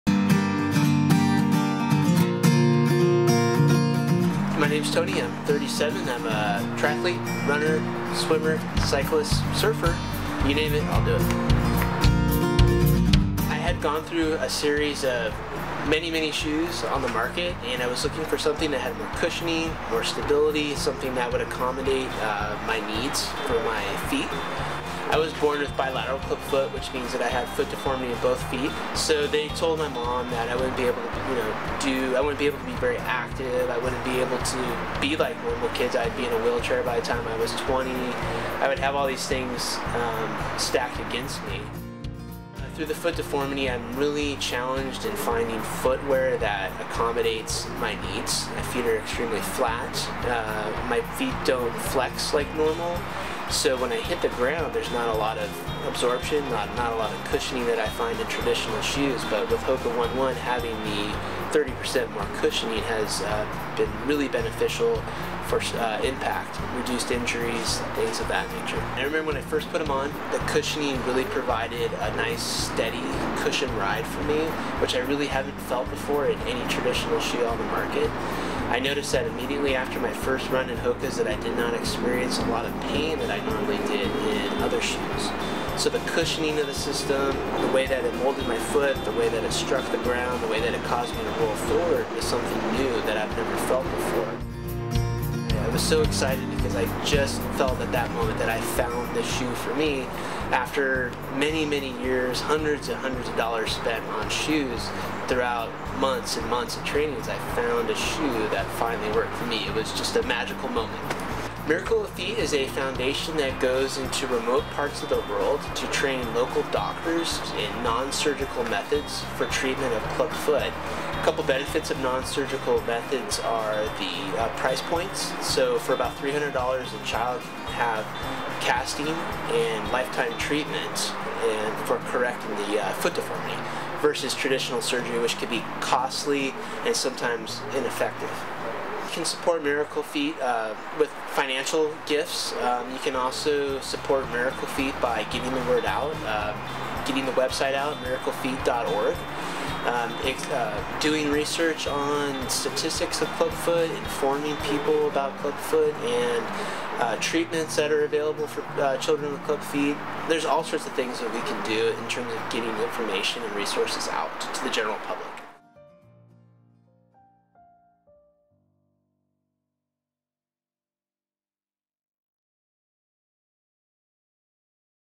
INTV TONY SPINETO
Listen to what Tony Spineto has to say about his work with the Miracle Feet Foundation and WHY he chooses to run in Hoka One One.